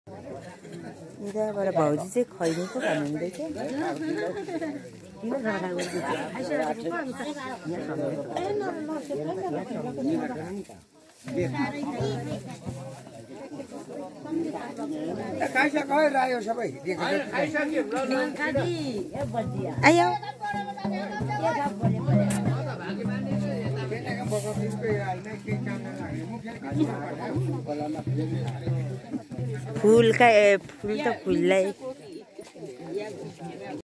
मारुनी नाच
ताया रेक बहादुर दुमी मुरहको चिर शान्तिको नाममा